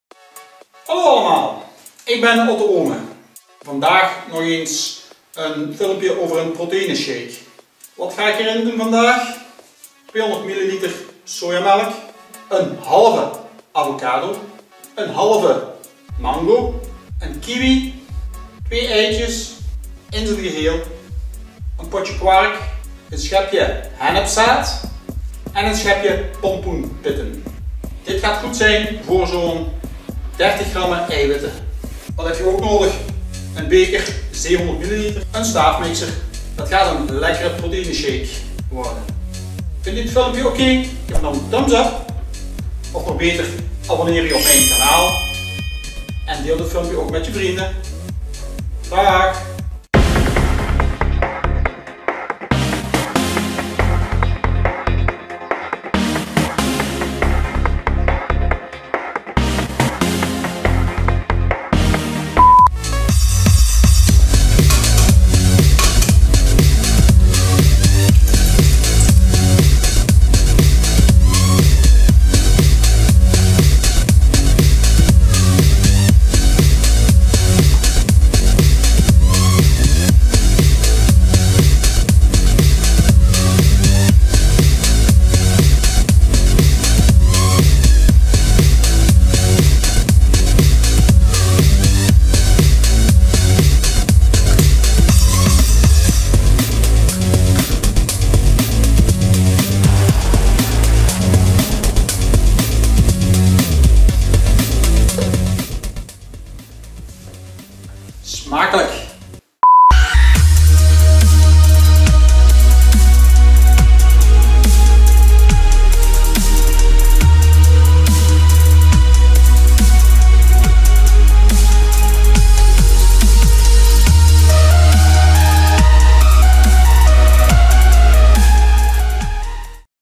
Proteïne Avocado Mango Shake - EiwitShake - Smoothie
Ingredienten
200ml Alpro Soya Melk
1/2 Avocado
1/2 Mango
1 Kiwi
2 Eieren
1 Potje kwark
1 Schepje hennepzaad
1 Schepje Pompoenpitten

Benodigdeden
Beker: 700ml
Staafmixer

Muziek:
teknoaxe

Outro:
teknoaxe